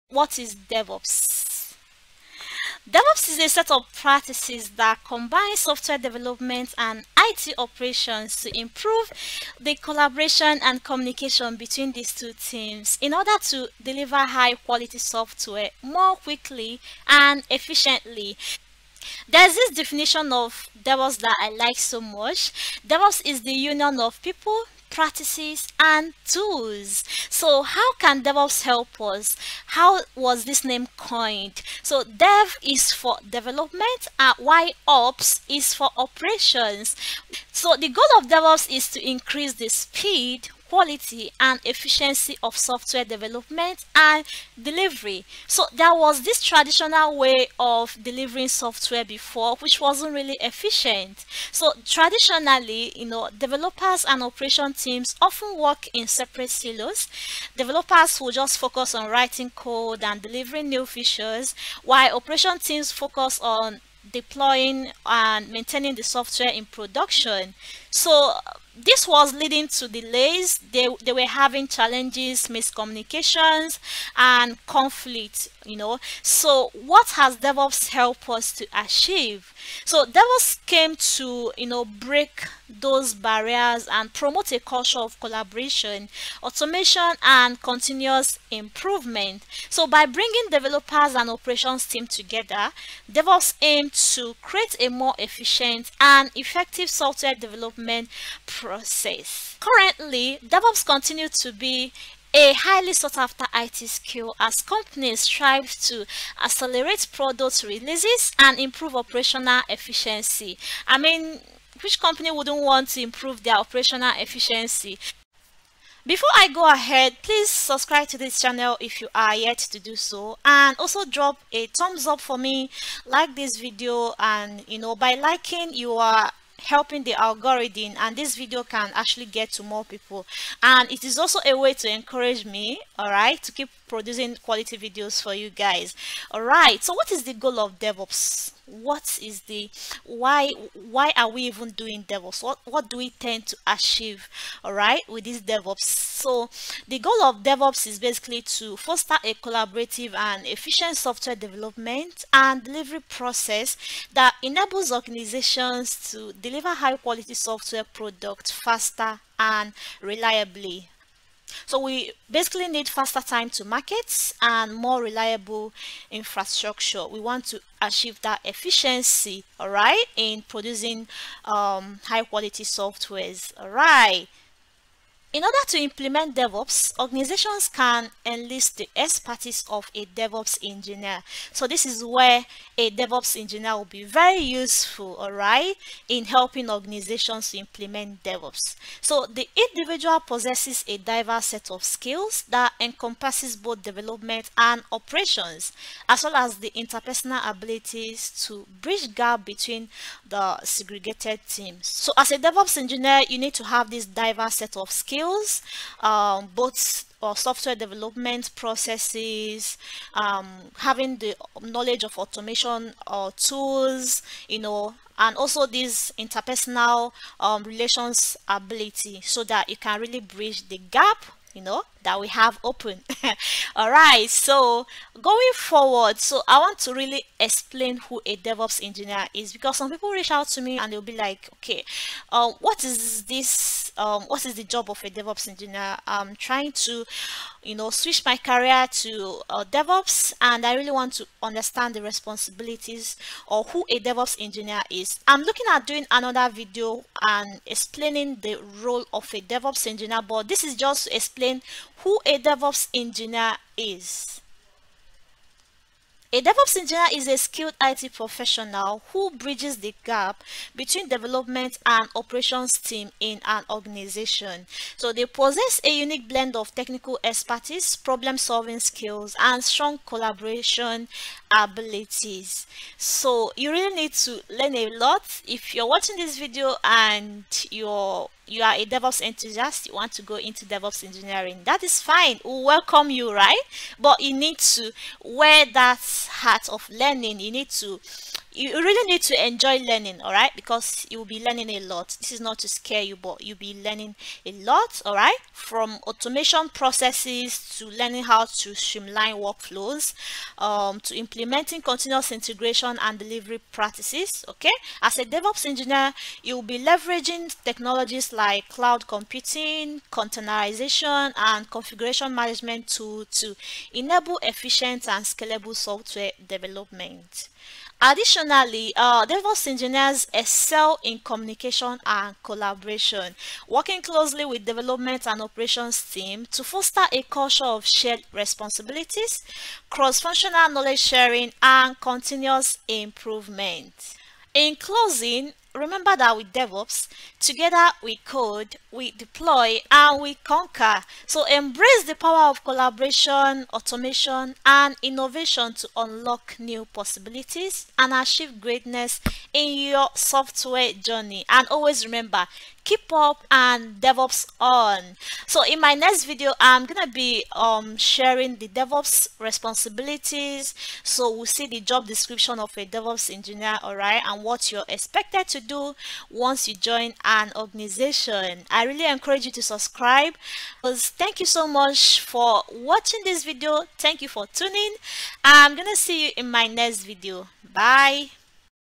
What is DevOps? Who is a DevOps Engineer? | Full Explanation of DevOps
Dive into the world of DevOps and discover how this powerful approach is transforming software development and delivery.

In this short video, I have explained what DevOps is as well as who a DevOps Engineer really is.
Explore the collaborative and efficient practices that bridge the gap between development and operations teams.